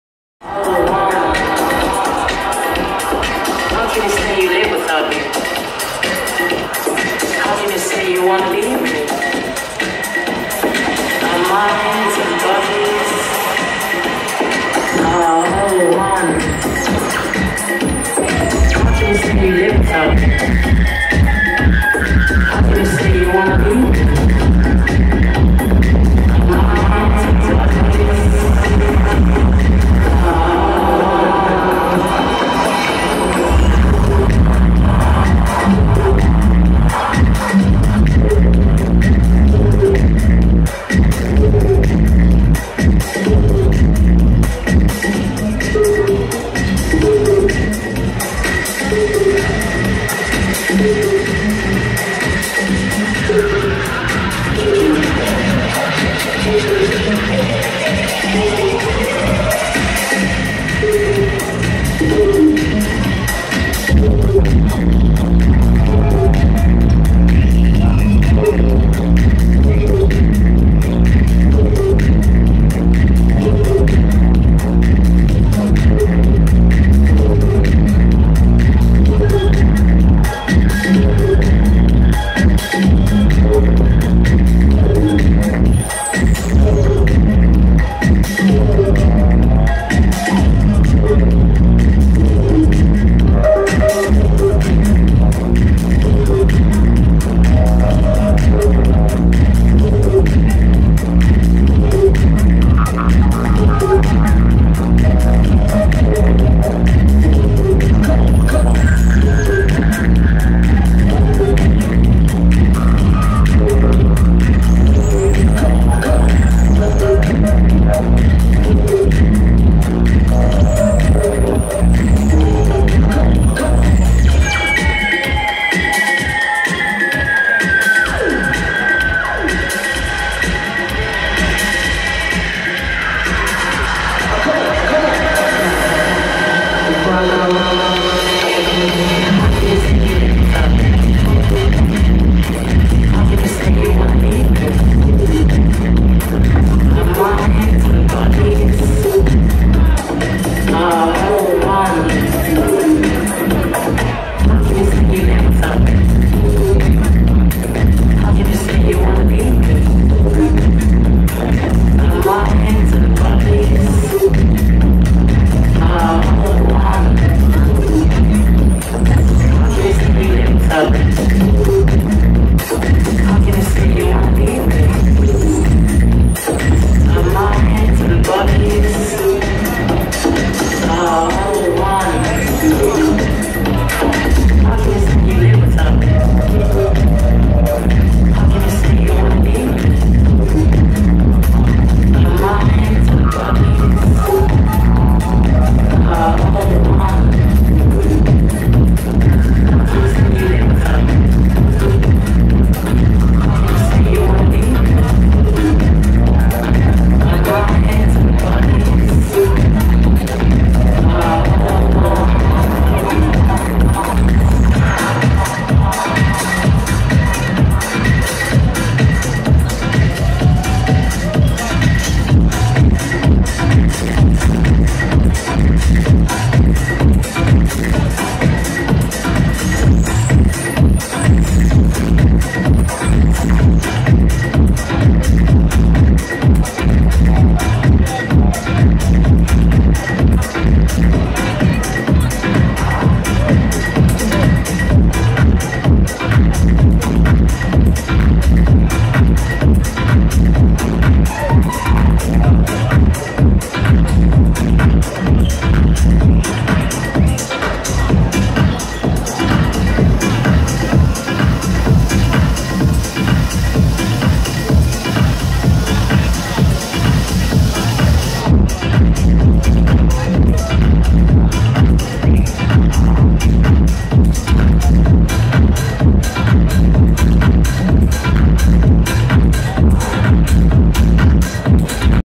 The Martinez Brothers B2B Seth Troxler 12 Hours Set @ Sunwaves 21 - Romania 2017 - [2]
Sunwaves Festival, 21st Edition. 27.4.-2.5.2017 @ Mamaia North Beach, Romania.